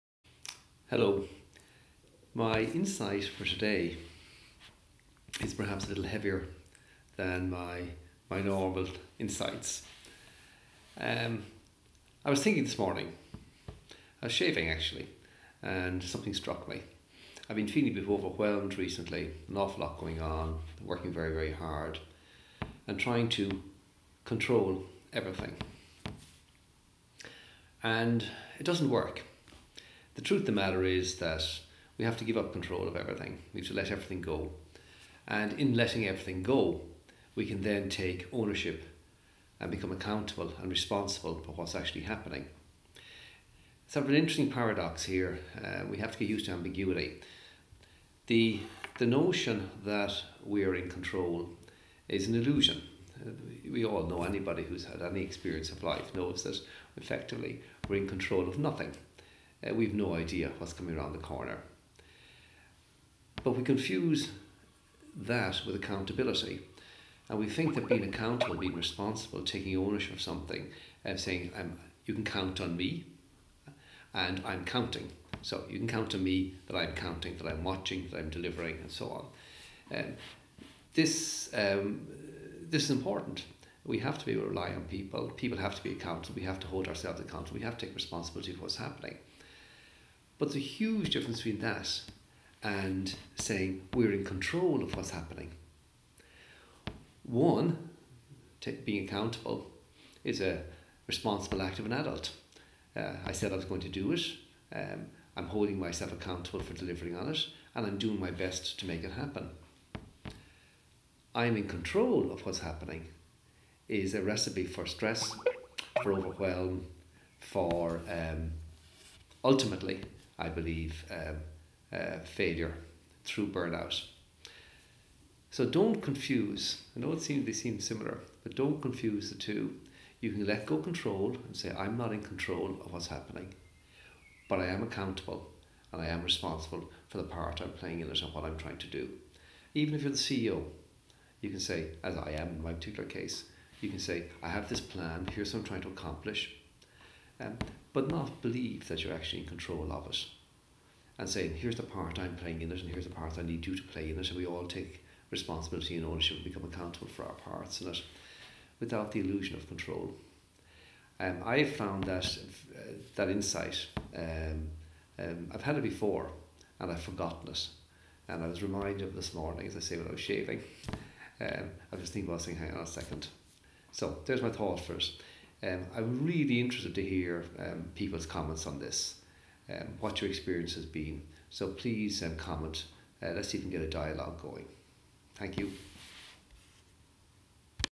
Out of Control
My insight for today is perhaps a little heavier than my normal insights.

As I was shaving this morning something struck me. I've been feeling a bit overwhelmed recently. I've an awful lot going on and I've been working very hard, and I realised that I've been trying to control everything.

It doesn't work.

The truth of the matter is that we can't control everything. We have to let everything go. In letting everything go we can then take ownership and become accountable and responsible for what's actually happening.

It's sort of an interesting paradox here. We have to get used to ambiguity. The notion that we're in control is an illusion.

We've no idea what's coming around the corner.

Being accountable is important, but there's a huge difference between that and saying we're in control of what's happening. Being accountable is the responsible act of being an adult. It's doing what you said you would do. When we think we're in control of what's happening it's a recipe for stress, for overwhelm and ultimately failure through burnout. I know they seem similar but don't confuse the two. We can let go of control but still be responsible and accountable.

I've had this insight before but I'd forgotten it. It's time to let go of the illusion of control.

I'd be really interested to hear people's thoughts and comments on this. Let's see if we can get a dialogue going.